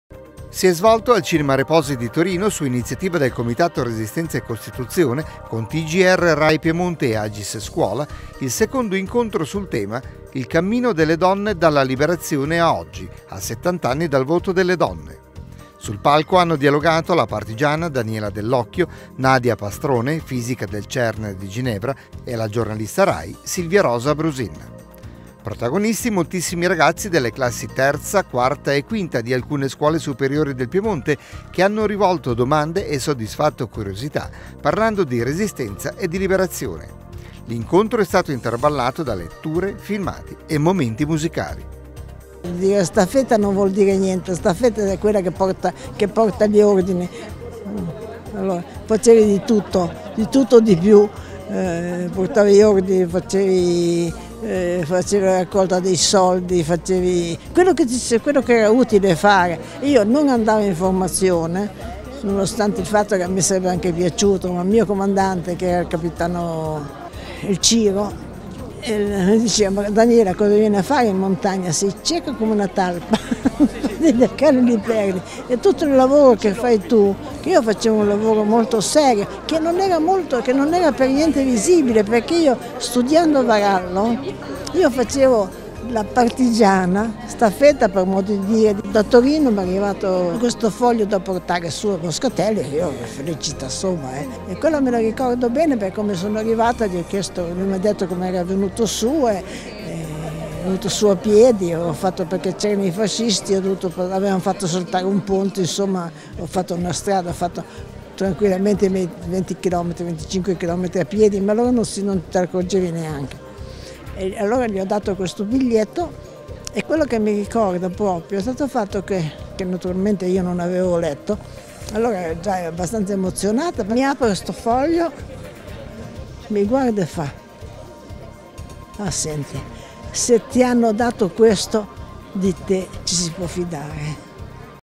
Partigiane di ieri e donne di oggi
Si è svolto al Cinema Reposi di Torino, su iniziativa del Comitato Resistenza e Costituzione, con TgRai Piemonte e AGIS Scuola, il secondo incontro sul tema: "Il cammino delle donne dalla Liberazione a oggi", a 70 anni dal voto alle donne. Sul palco hanno dialogato la partigiana Daniela Dell’Occhio, Nadia Pastrone, fisica del Cern di Ginevra, e la giornalista Rai Silvia Rosa Brusin. Protagonisti moltissimi ragazzi delle classi III, IV e V di alcune scuole superiori del Piemonte che hanno rivolto domande e soddisfatto curiosità parlando di Resistenza e di Liberazione. L’incontro è stato intervallato da letture, filmati e momenti musicali.